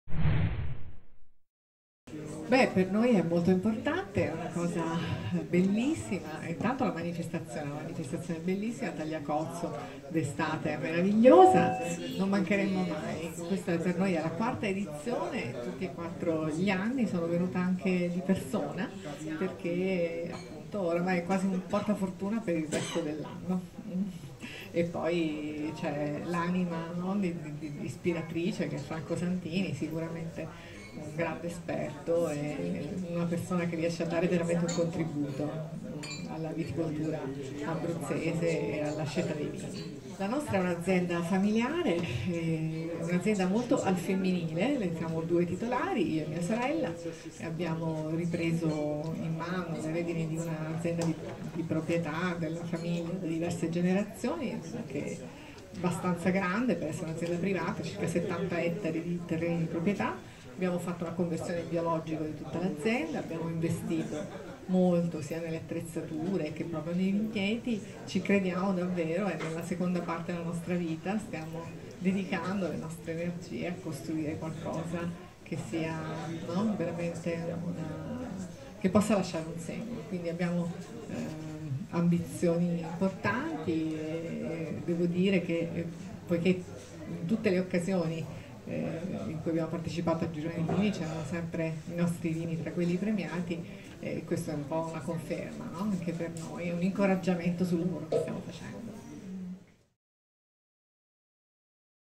Gironi divini, la parola ai protagonisti: Carla Perrucci della Tenuta Tre Gemme
Gironi Divini nasce 9 anni fa con la volontà di realizzare un evento del vino di qualità in Abruzzo, ispirato alla figura di Dante Alighieri e al suo legame storico con la città di Tagliacozzo. Nel tempo è cresciuto e si è evoluto, divenendo, numeri alla mano, uno dei più importanti appuntamenti del vino in Abruzzo, con oltre 70 cantine aderenti e una giuria tecnica di esperienza internazionale. Degustazioni guidate organizzate da Franco Santini, direttore artistico della manifestazione, permettono ai partecipanti di venire a conoscenza di curiosità e dettagli del vino abruzzese che in pochi conoscono.